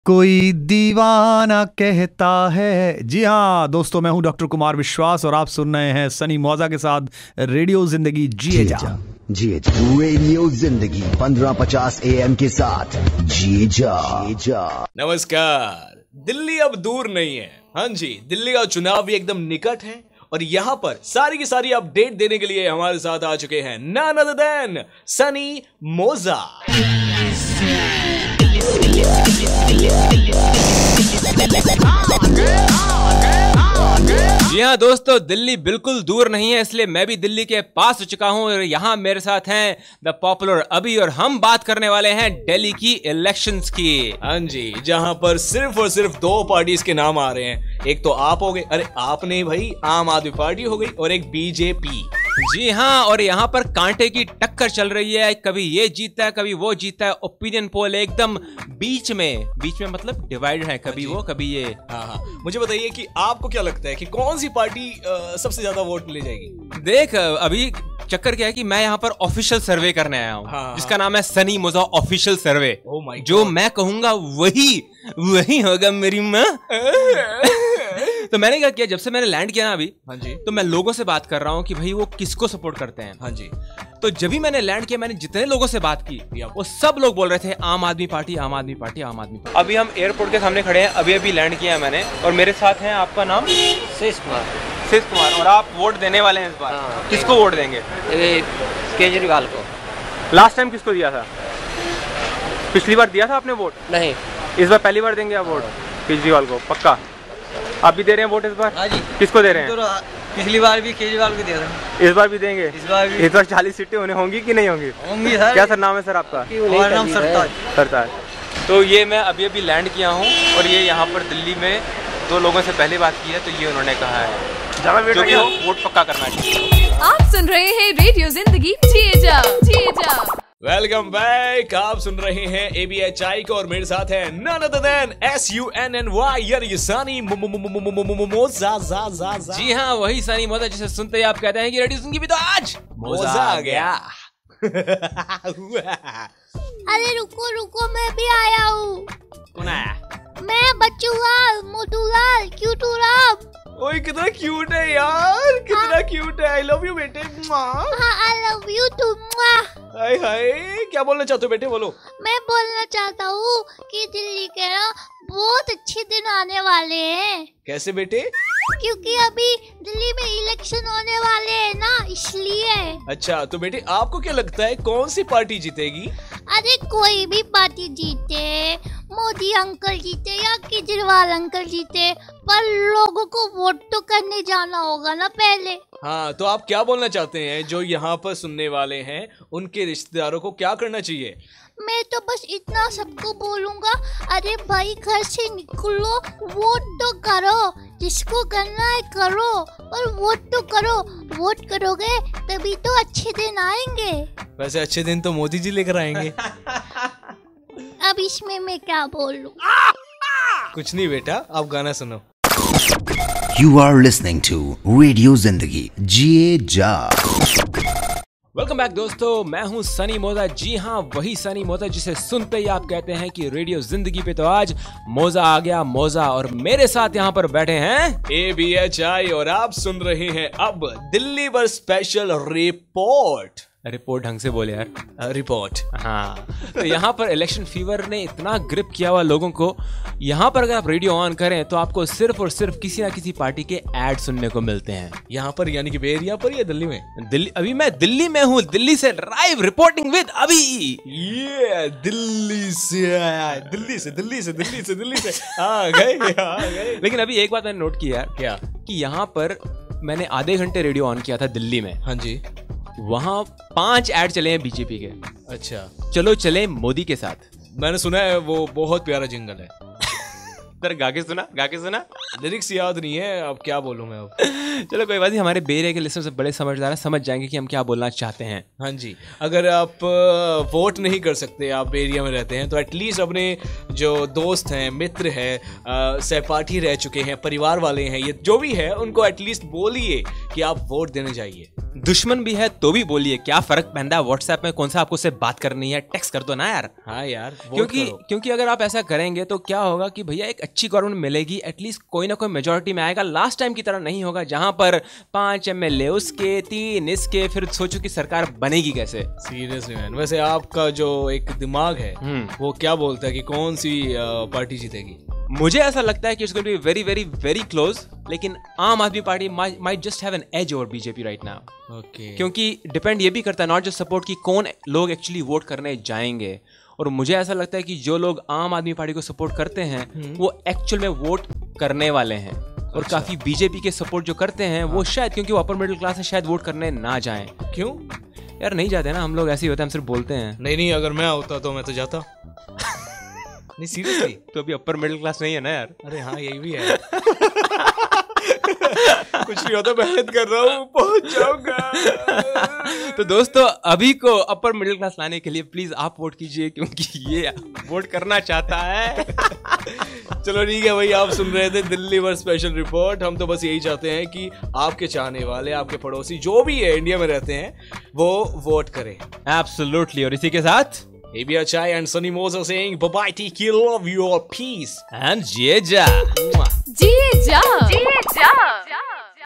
Delhi Elections 2015 Update - Sunny Moza & Abbhishek!
Live update from the battlefield - Delhi Elections 2015.
Sunny Moza and Abbhishek along with Bachhu-lal - only on Radio Zindagi.